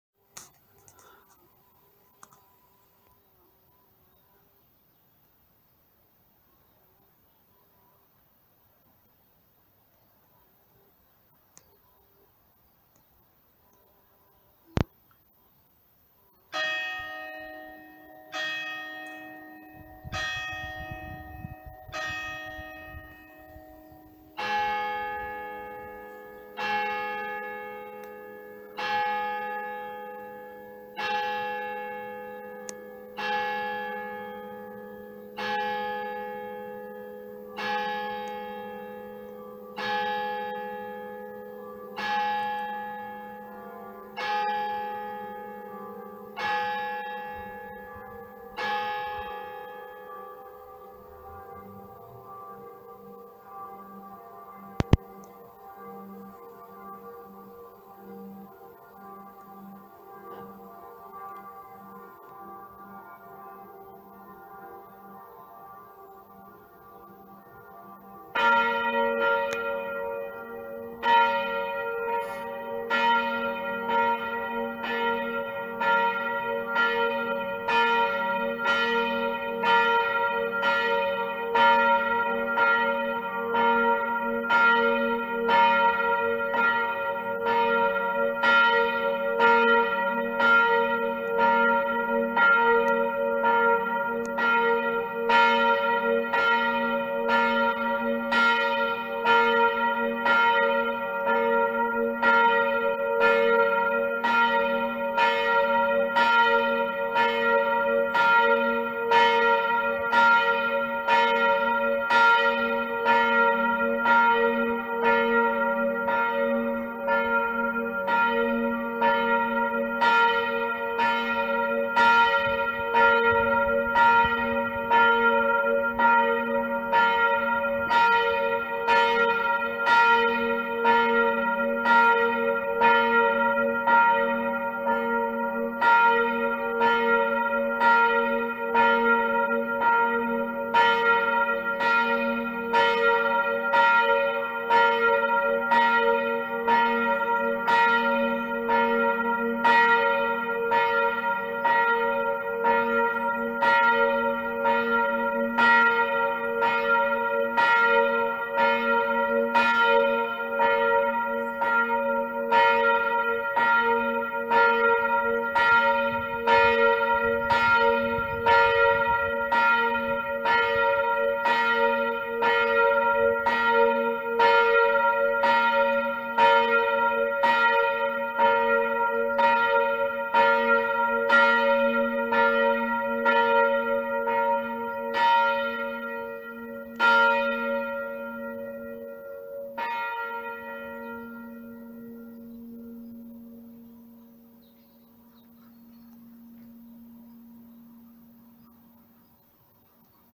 12 Uhr Läuten der evangelischen Auferstehungskirche Ulm Böfingen
Bevor diese Kirche gebaut wurde stand ihr Name Auferstehungskirche fest. Diese Kirche wurde von 1964 bis 1966 gebaut. Der Turm ist schlank, hat eine Höhe von 40 Metern und beinhaltet fünf Glocken. Unter diesem Link kann man noch mehr erfahren, wo ich einzelne Informationen zu dieser Kirche herausgepickt habe Es gibt 4 Schläge auf eine der kleineren Glocken dafür, dass die Stunde voll ist. Dann gibt es 12 Schläge auf eine der größeren Glocken für 12 Uhr. Nach einer kurzen Pause beginnt eine der mittleren Glocken für 2 Minuten zu läuten. Während des Videos kann man das Angelusläuten der katholischen Kirche "Zum Guten Hirten" hören, weil diese Kirche mit 2 Glocken zum Angelus läutet.